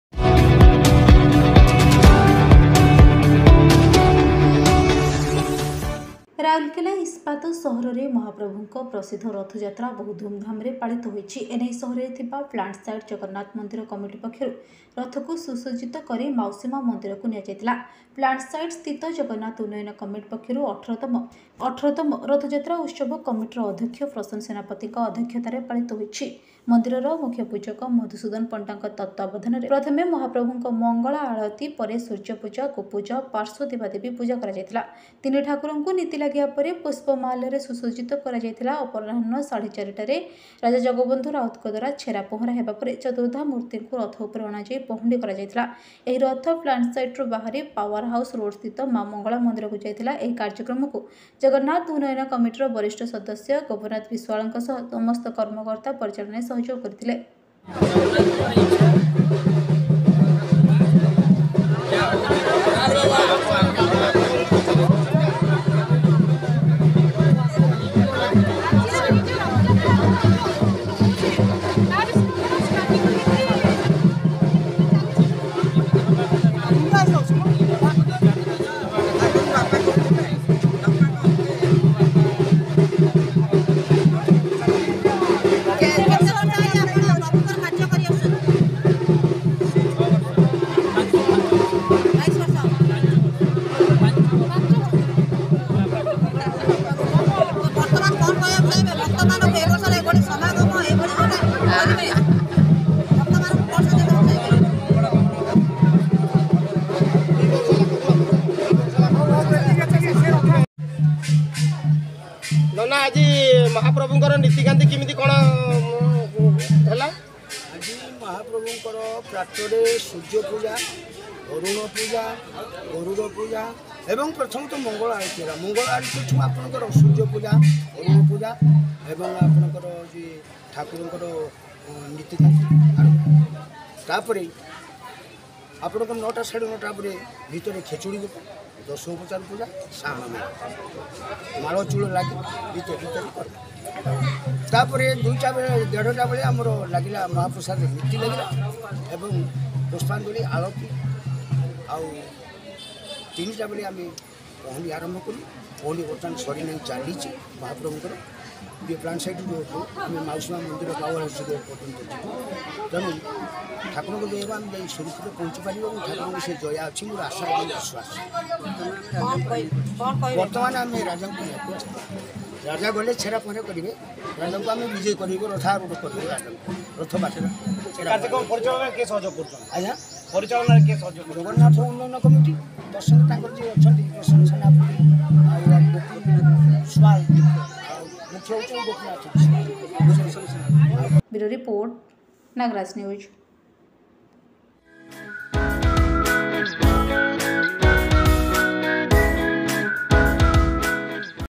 ରାଉରକେଲା ସହରରେ ରଥଯାତ୍ରା ପାଳିତ#nagrajnews
The famous chariot ride of the Lord is celebrated in the steel city of Rourkela. The chariot was taken to the Mausi Maa temple by the Jagannath Temple Committee, a plant site in the city. The 16th Rath Yatra Utsav Committee, chaired by Prasanna Senapati, Chairman of the Plantation Site-based Jagannath Upliftment Committee, celebrated the occasion. Under the supervision of Madhusudan Panda, the chief priest of the temple, the first worship of the Lord was followed by the worship of the Sun, the worship of the Gopuja, and the worship of the side deities. The three Thakurs were adorned with wreaths after the policy was adopted. At around 4.30pm, Chaturdhamurthy was brought on a chariot and beaten by King Jagbandhu Raut. The chariot left the plant site and went to the Maa Mangala temple on Powerhouse Road. Gopinath Bishwal, a senior member of the Jagannath Upliftment Committee, assisted in organizing the event.